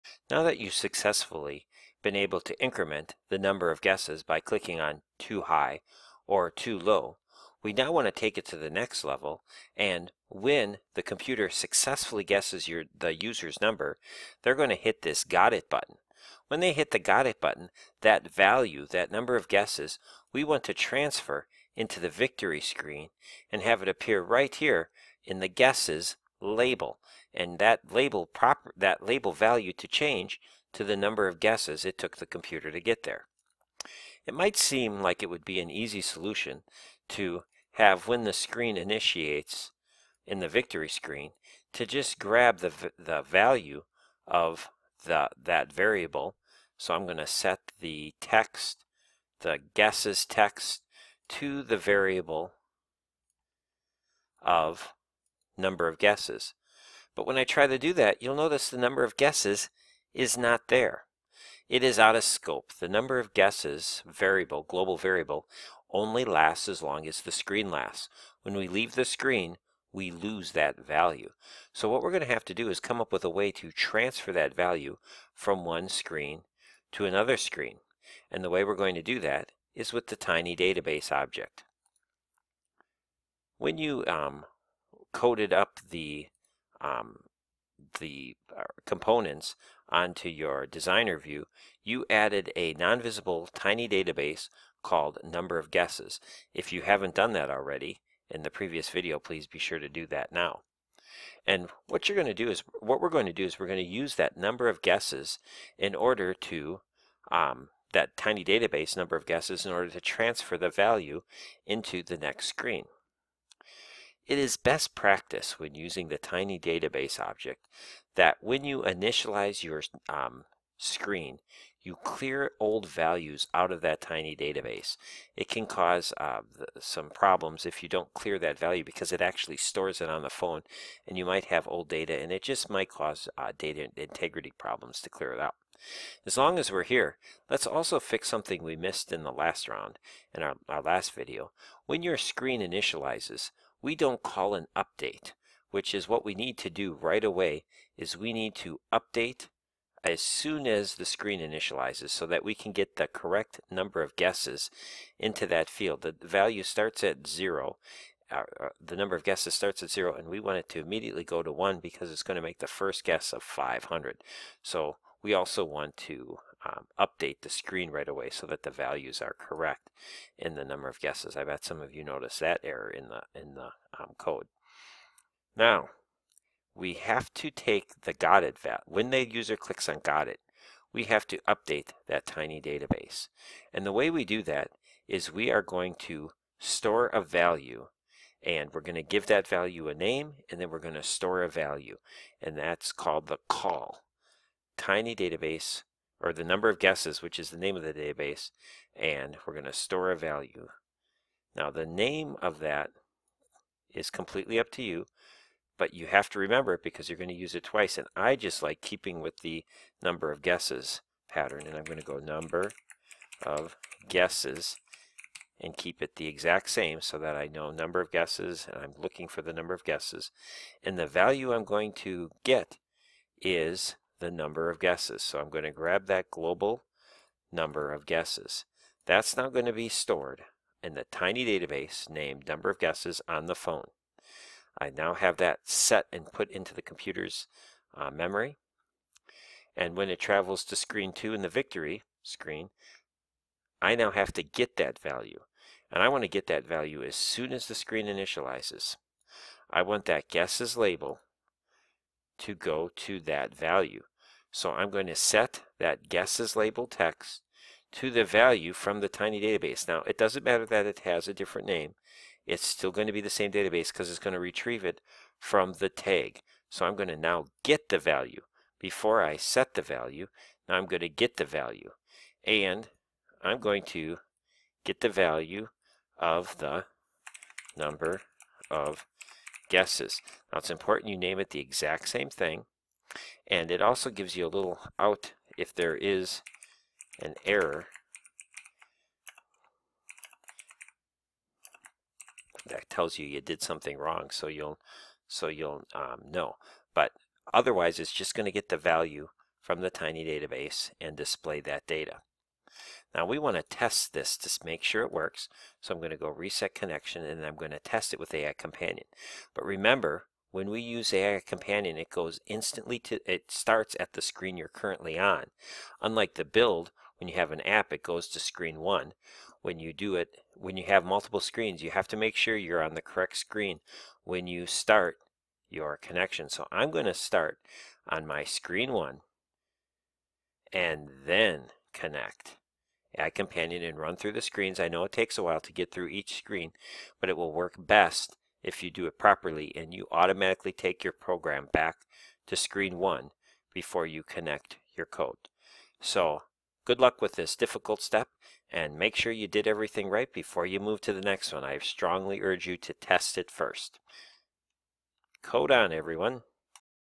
NumberWizards - TinyDB
This video shows you how to install the TinyDB on your phone so that you can move values from one screen to another.